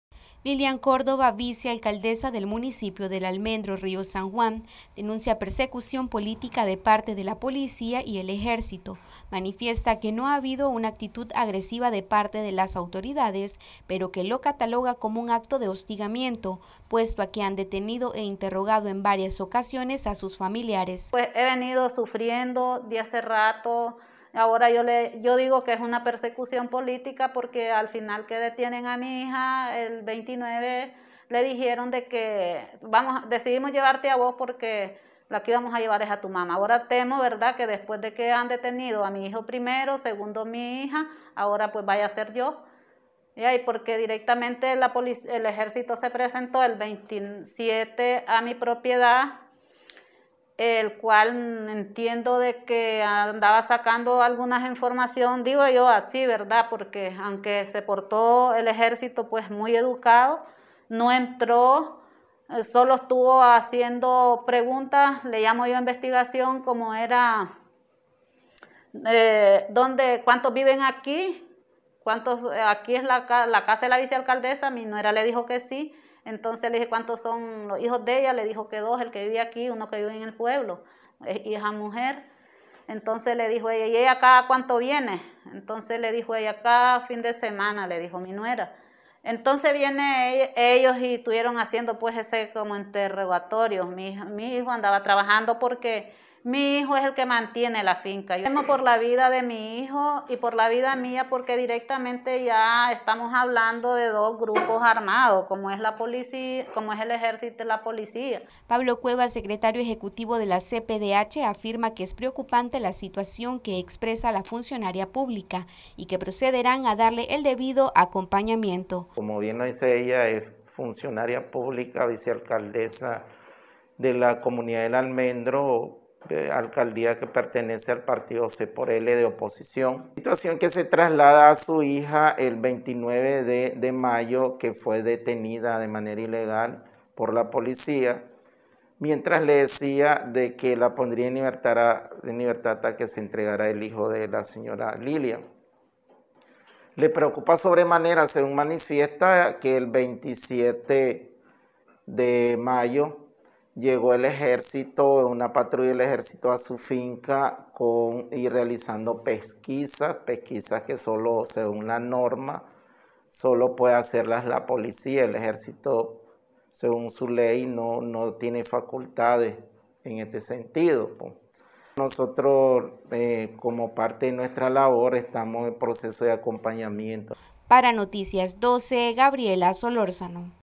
Vice alcaldesa del almendro denuncia asedio de parte de autoridades a sus familiares
NosResistimosAlEmbargo Vice alcaldesa del almendro que pertenece al partido político ciudadanos por la libertad denuncia asedio de parte de autoridades a sus familiares